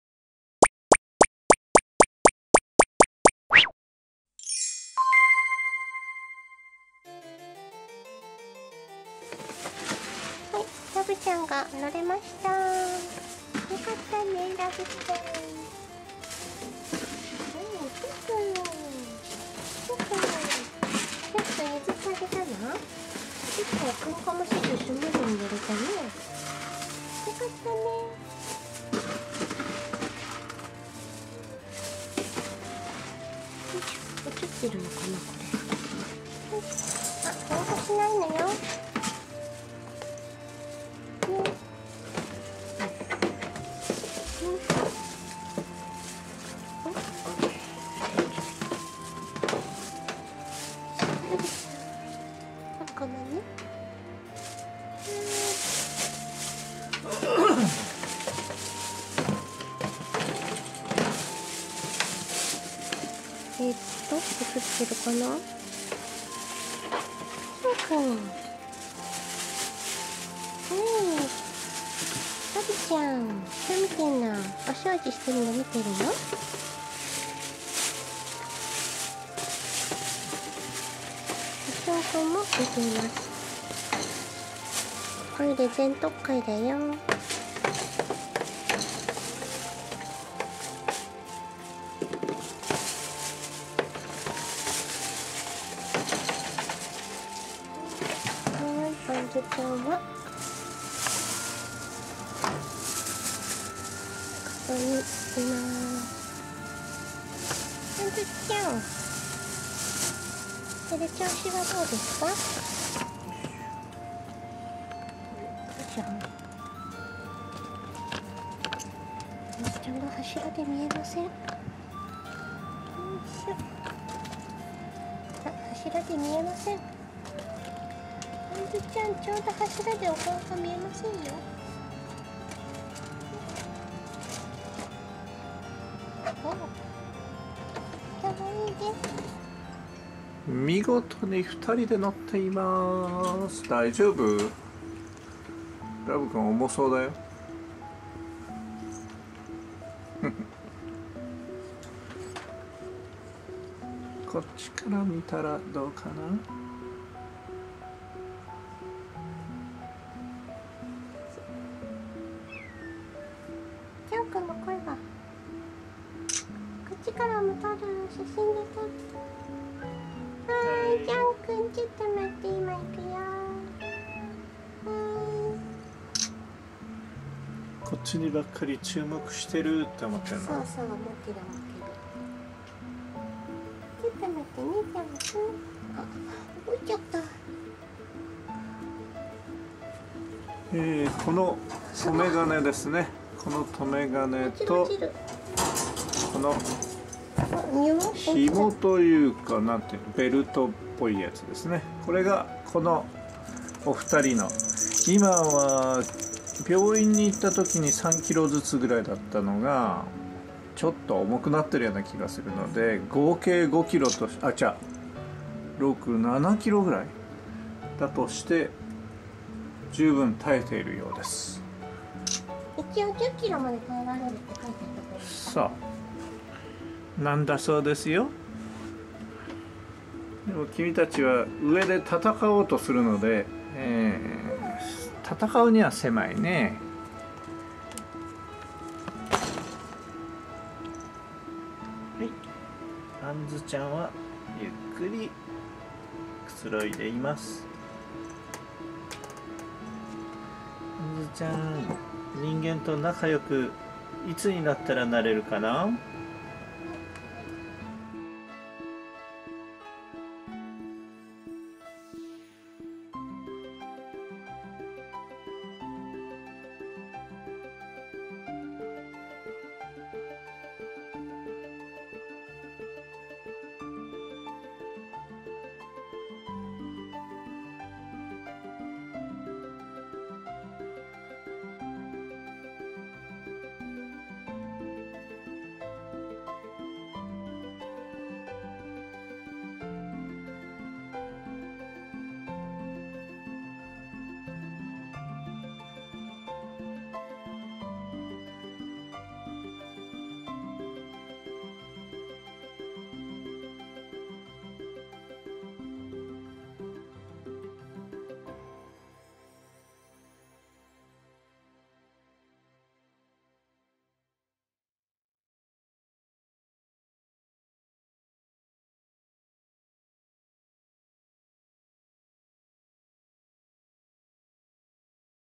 【Jean & Pont 859】ハンモックに2匹で乗ったラブくん＆しおんくん 2017/12/1 ジャン ポン ラブ しおん あんず
ハンモックをプレゼントしてくれた†姫組†さんのアドバイスでピーンと張ってみたら見事に2匹で乗ったラブくんとしおんくん。
可愛い姿で癒してくれます！
強度もばっちり余裕でした。

†姫組†（商品の購入はメッセージを送信で）